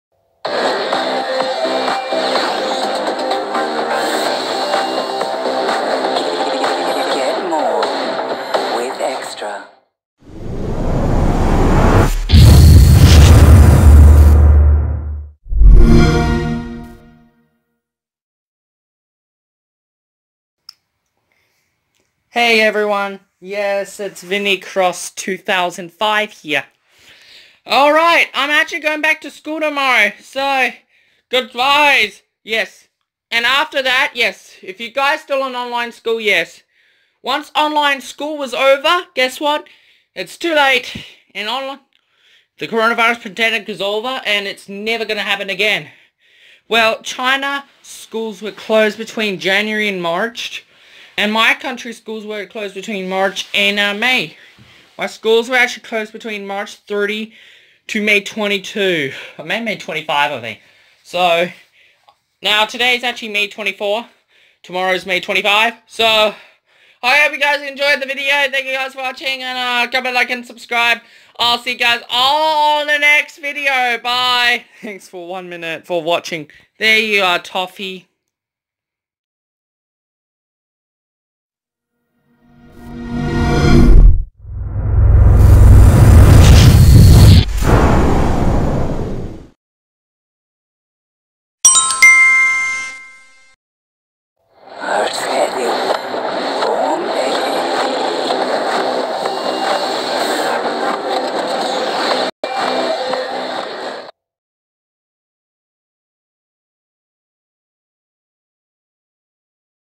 I'm going back to school tomorrow! 😀
My school has been closed from March 30 to May 22, 2020.